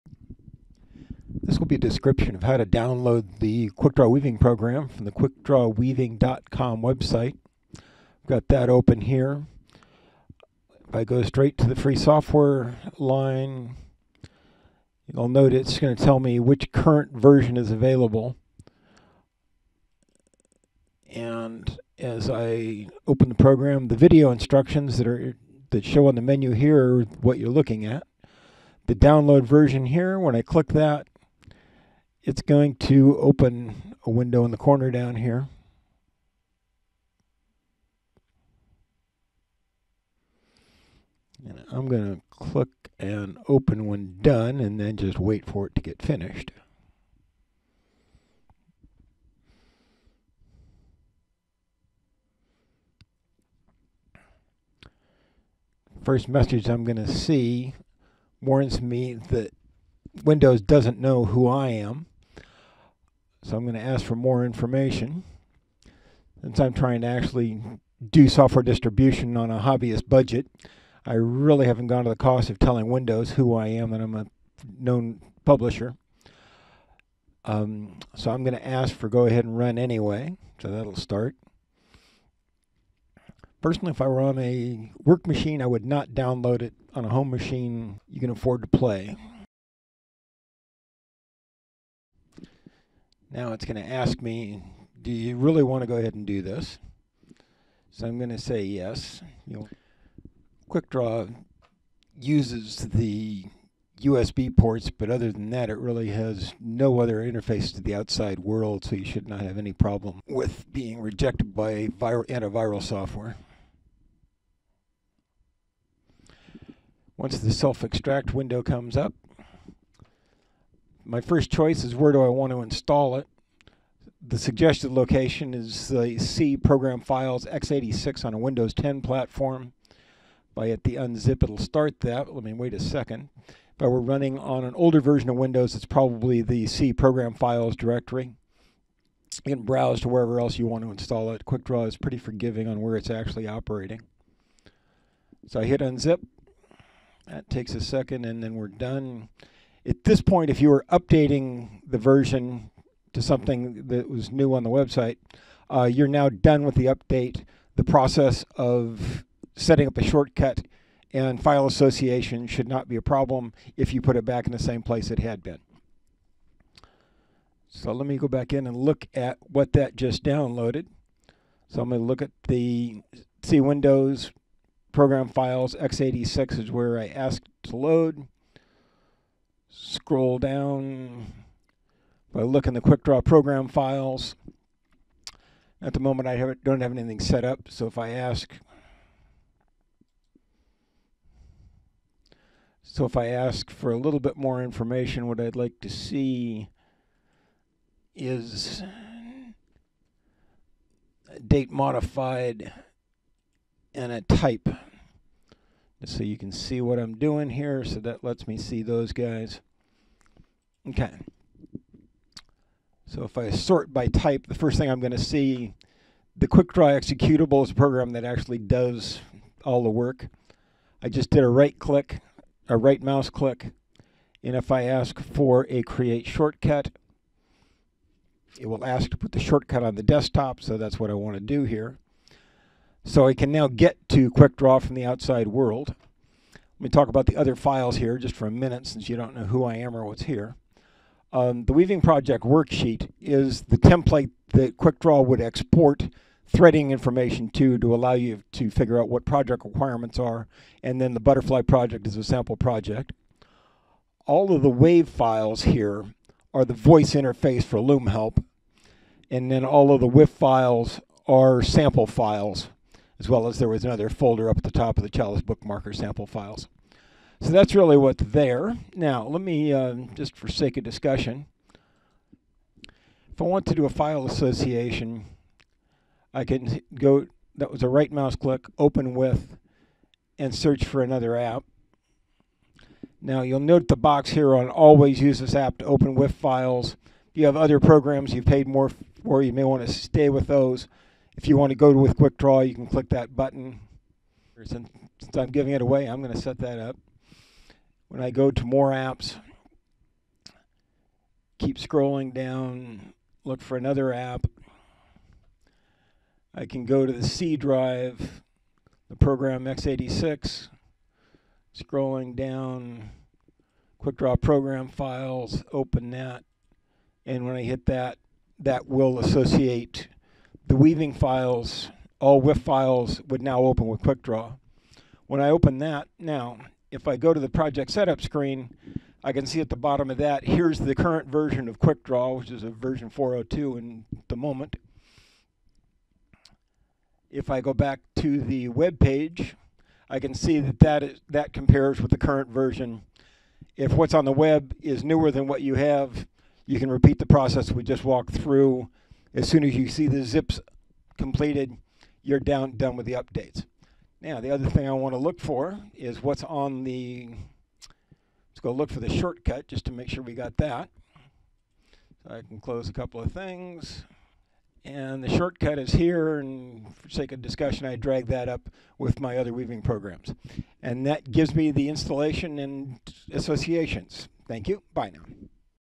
Quickdraw Weaving 1.2 Downloading & Installing the Quickdraw program
Free software download from the "QuickdrawWeaving.com" website.
There is also a "Quickdraw Weaving Users" Group on Facebook which talks about status & pending changes as Quickdraw evolves